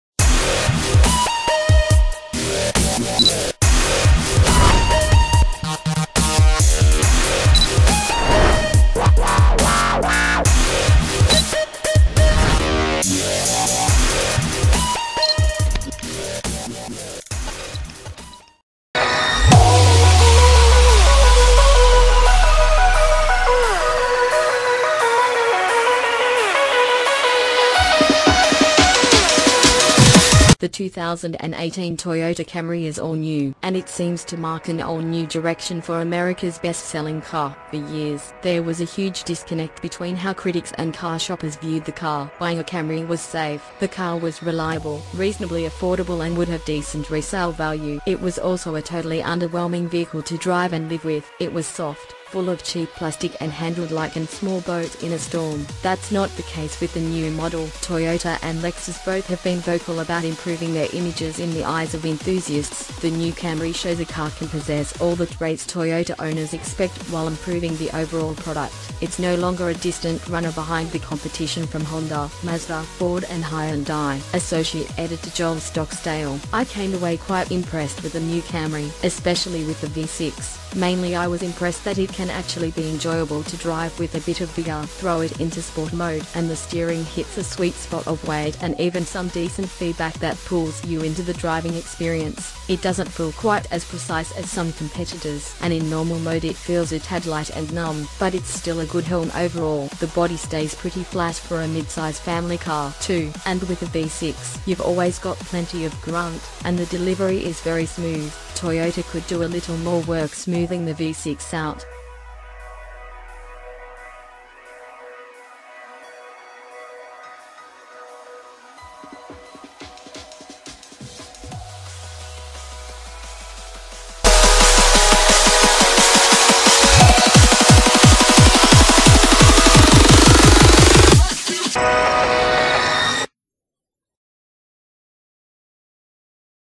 WOW AMAZING!!2018 Toyota Camry XLE V6 Overview
WOW AMAZING!!2018 Toyota Camry XLE V6 Overview - The 2018 Toyota Camry is all new, and it seems to mark an all-new direction for America's best-selling car. For years, there was a huge disconnect between how critics and car shoppers viewed the car. Buying a Camry was safe. The car was reliable, reasonably affordable and would have decent resale value. It was also a totally underwhelming vehicle to drive and live with. It was soft, full of cheap plastic and handled like an small boat in a storm.

Popular Post !